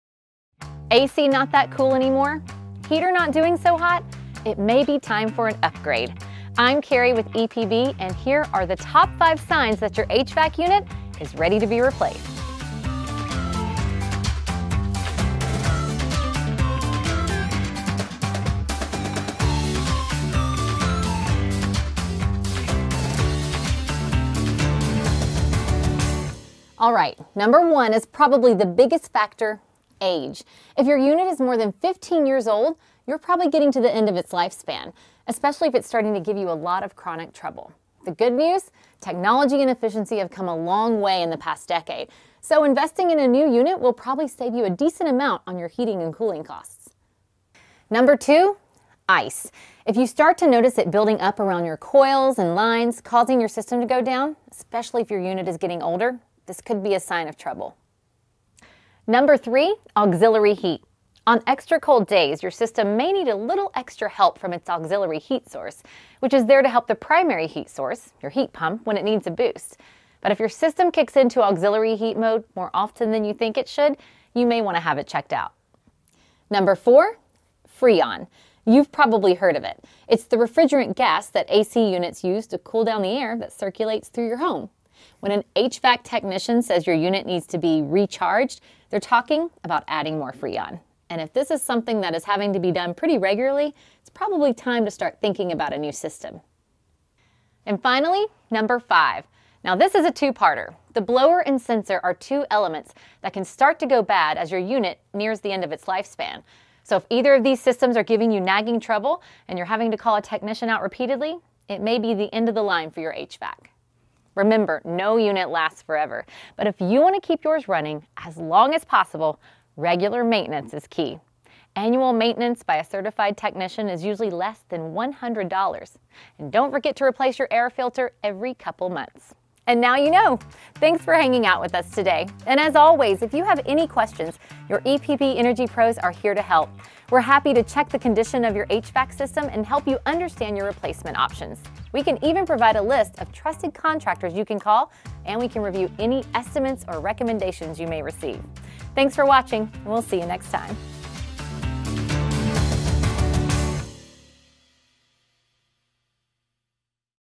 HVAC Replacement | EPB Energy Pros
At home with the EPB Energy Pros

Learn everything you need to know about replacing your heating and air unit!